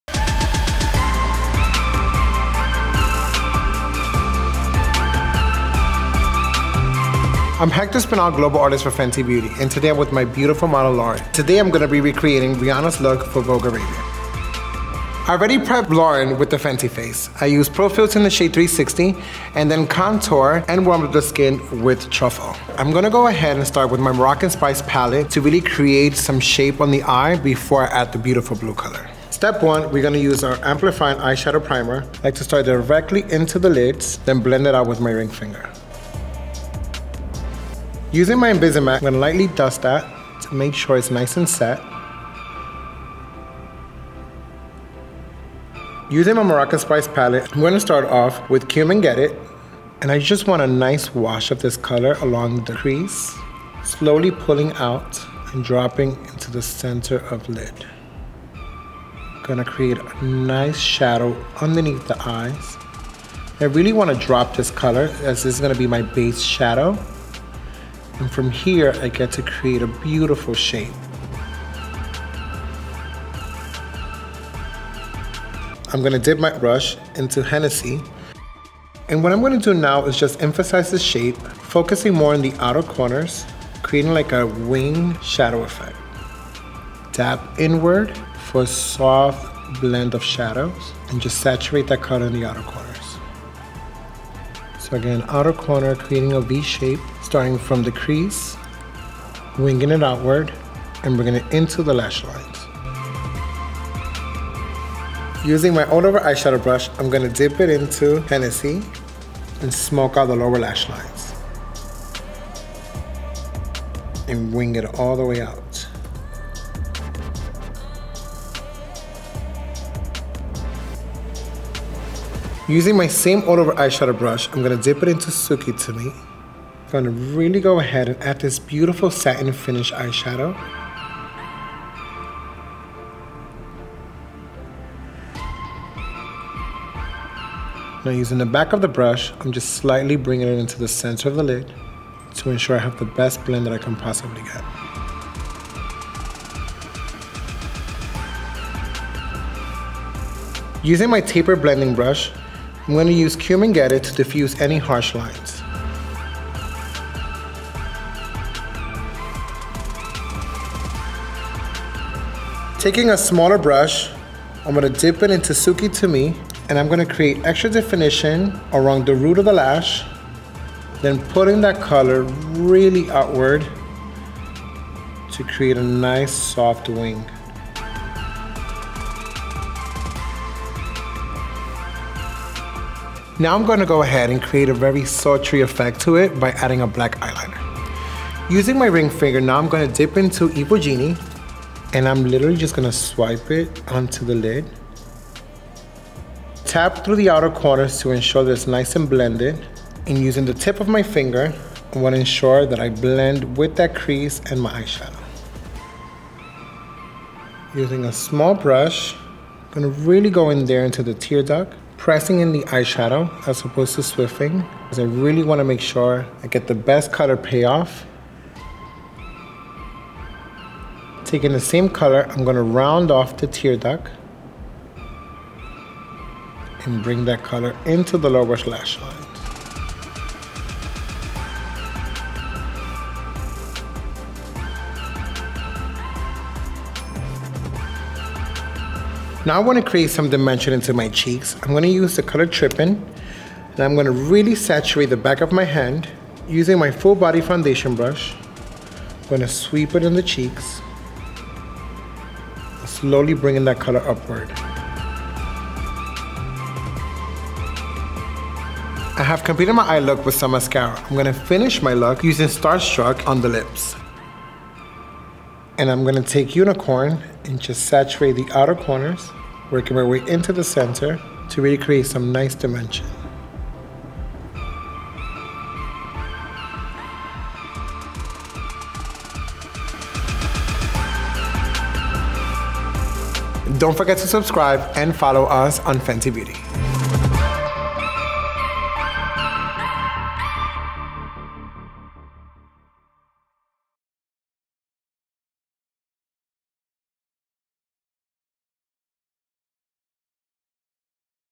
GET THE LOOK: RIHANNA VOGUE ARABIA | FENTY BEAUTY
Watch Fenty Beauty Global Makeup Artist, Hector Espinal, recreates Rihanna's iconic Vogue Arabia cover look using our Moroccan Spice Palette.

Fenty Beauty is 100% cruelty free.